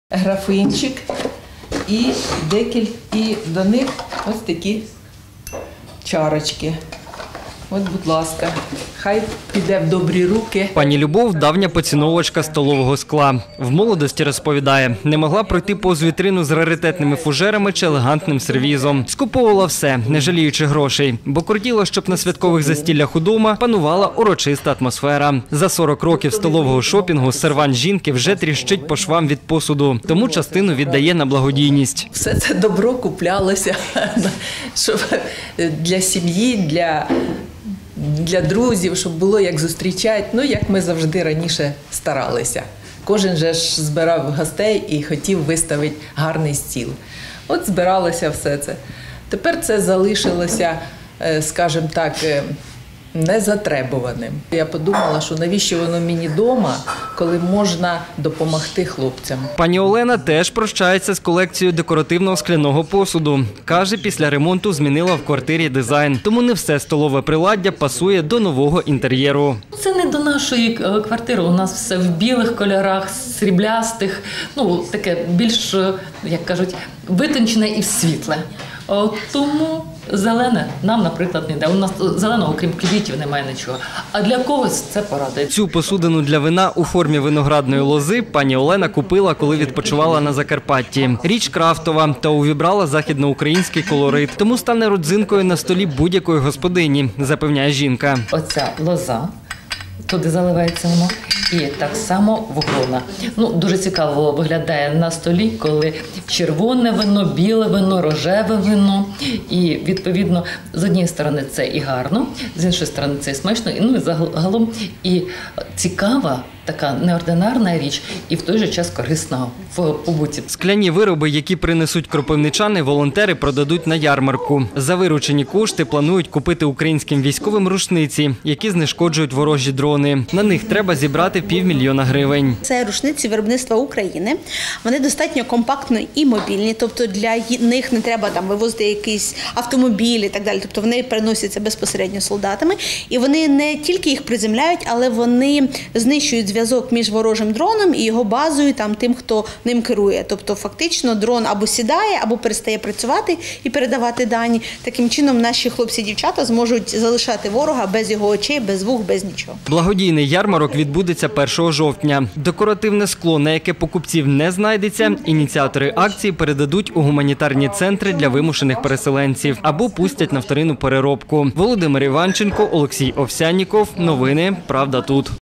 У Кропивницькому волонтери продаватимуть вироби зі скла, щоб заробити гроші на зброю для ЗСУ
Нині збирають келихи, графини, тарілки, столові сервізи та інше непотрібне скляне приладдя. Благодійний розпродаж волонтери проведуть 1 жовтня. На виручені кошти планують купити українським військовим рушниці, які знешкоджують ворожі дрони. Треба 500 000 грн. Декоративне скло, на яке покупців не знайдеться, ініціатори акції передадуть гуманітарним центрам для вимушених переселенців або пустять на вторинну переробку.